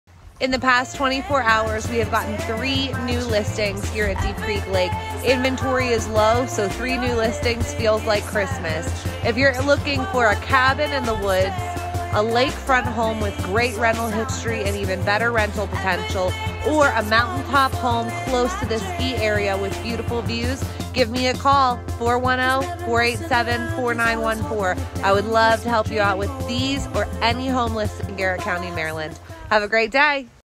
New Deep Creek Lake Homes for Sale
This week, three new listings hit the market at Deep Creek Lake! Inventory is so low right now, so three new listings to work with is pretty wonderful. If you're looking for a home at Deep Creek, let me know! I'll get you info on listings as soon as they hit the market, provide video tour, answer questions, and get you into your dream home!